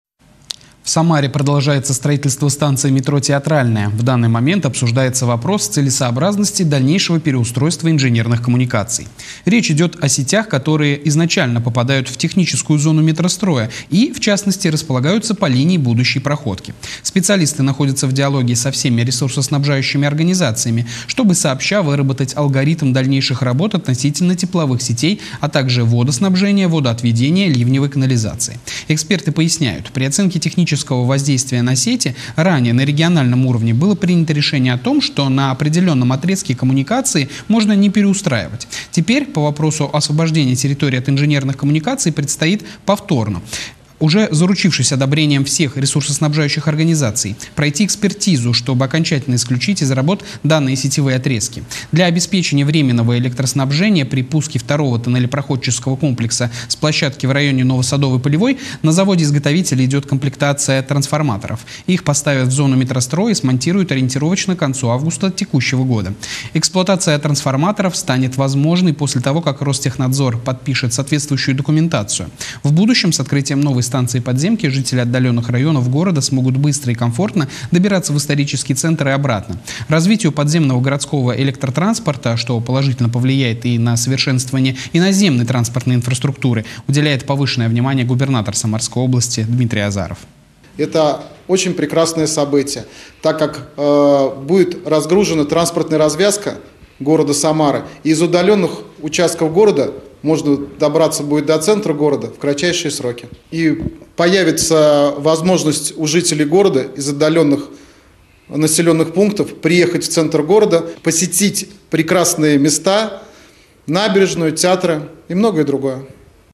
В Самаре продолжается строительство станции метро "Театральная"
В Самаре продолжается строительство станции метро "Театральная". В данный момент обсуждается вопрос целесообразности дальнейшего переустройства инженерных коммуникаций. Речь идёт о сетях, которые изначально попадают в техническую зону метростроя и, в частности, располагаются по линии будущей проходки. Специалисты находятся в диалоге со всеми ресурсоснабжающими организациями, чтобы сообща выработать алгоритм дальнейших работ относительно тепловых сетей, а также водоснабжения, водоотведения, ливневой канализации. Эксперты поясняют - при оценке технического воздействия на сети, ранее, на региональном уровне было принято решение о том, что на определенном отрезке коммуникации можно не переустраивать. Теперь по вопросу освобождения территорий от инженерных коммуникаций предстоит повторно, уже заручившись одобрением всех ресурсоснабжающих организаций, пройти экспертизу, чтобы окончательно исключить из работ данные сетевые отрезки.